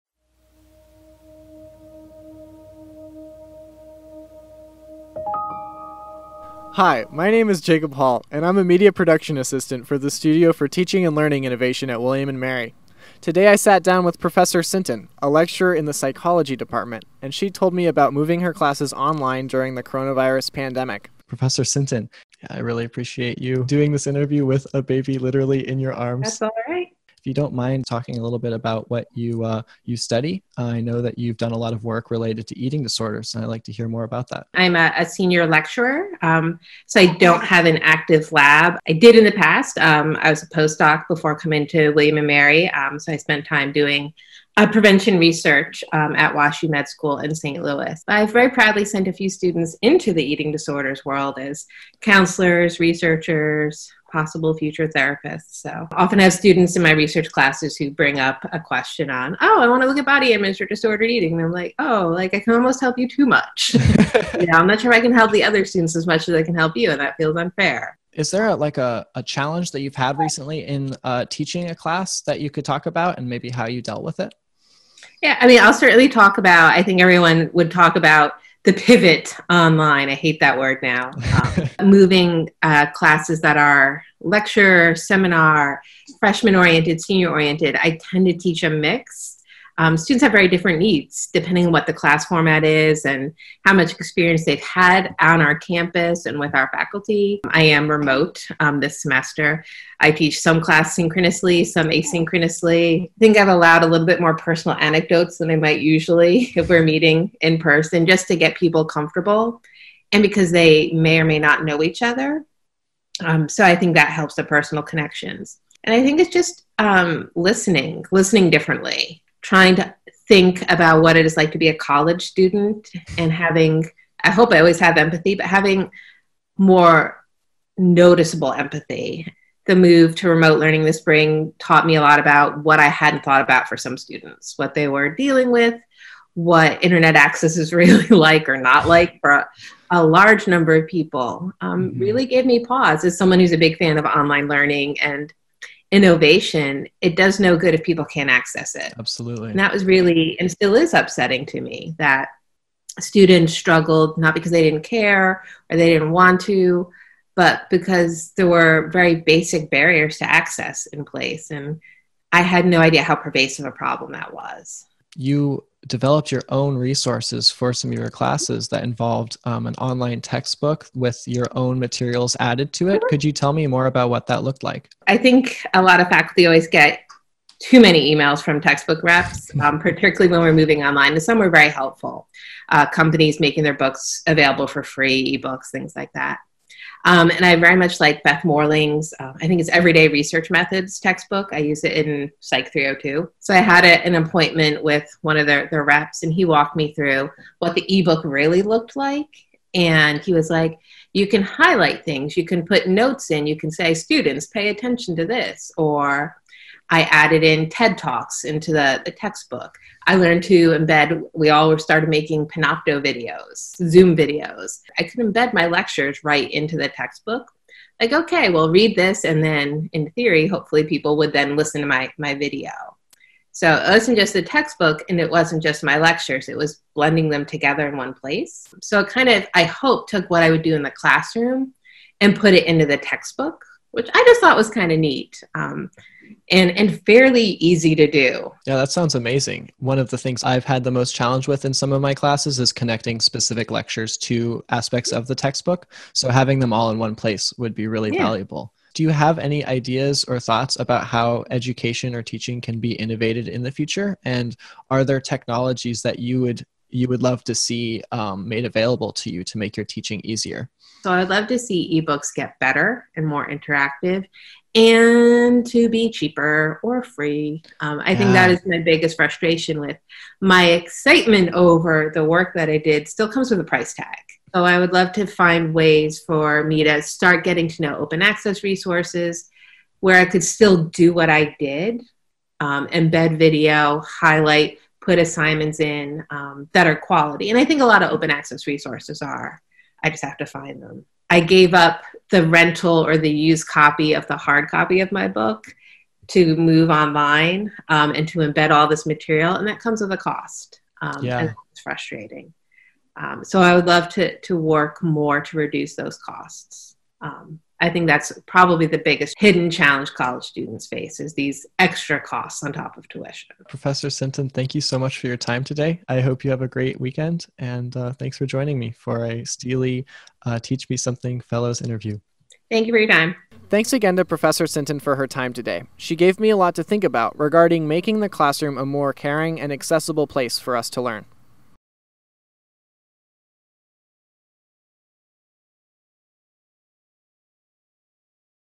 Meghan Sinton Miller: Pivot
W&M Sociologist Meghan discusses the benefits to giving students the opportunity to share their anecdotes and the power of listening.